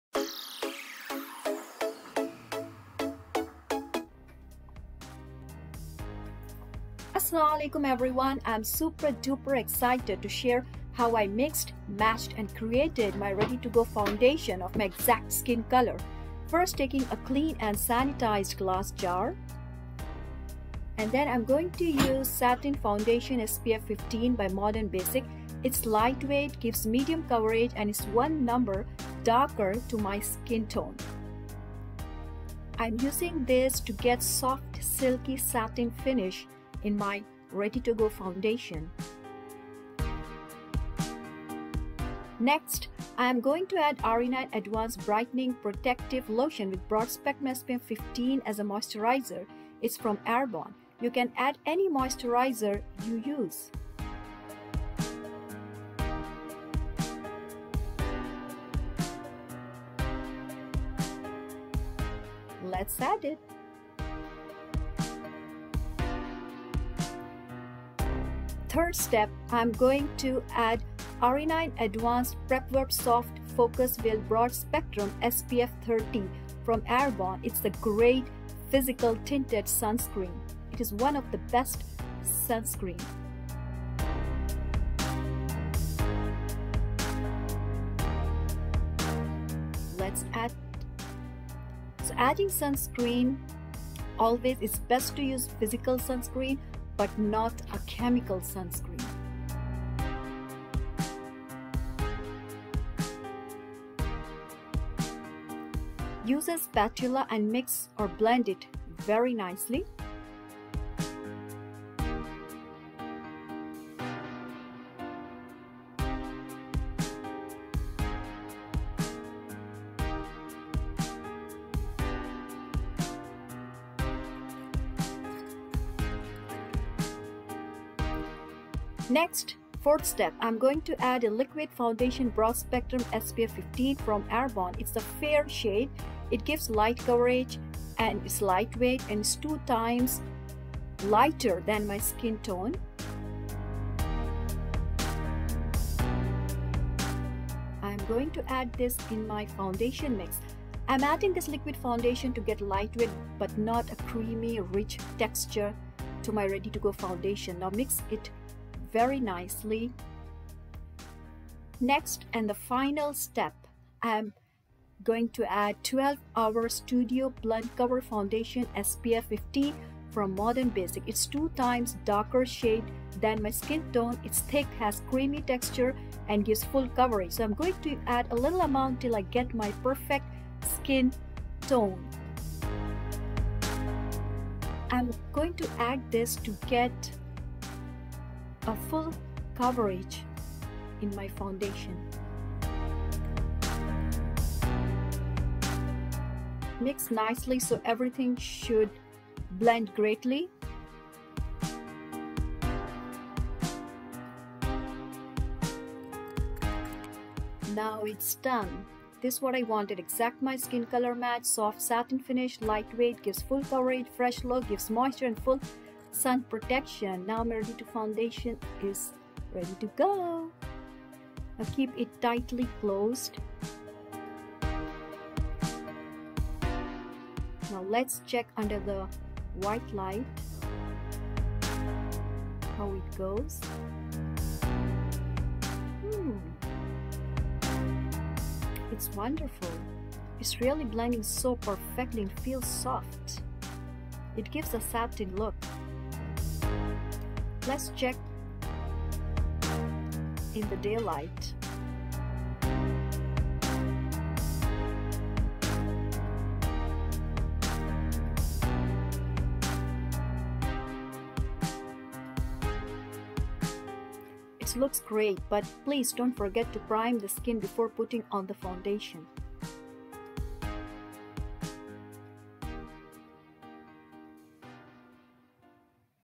How I MIXED, MATCHED and CREATED MY PERFECT COLOR FOUNDATION | Get READY in minutes | DAY MAKE-UP
Assalamu alaikum ! Amazing viewers, it's hard to find my exact foundation base. So
.  I mixed 3 different foundations from 2 different companies- Satin finish & 12-hr studio blend from Modern basic (wholesale only meant for certified estheticians, beauticians and beauty schools. Liquid foundation fair shade from Arbonne.
 . I even added sunscreen and moisturizer to my ready-to-go foundation.

So, please watch the whole video to find out how I created light weight, soft satin finish, full sunscreen protection, moisturized with full coverage.
-No more wasting time in the mornings, Easy, quick use, & ready to go.
- Make sure to prep & prime the skin before using it.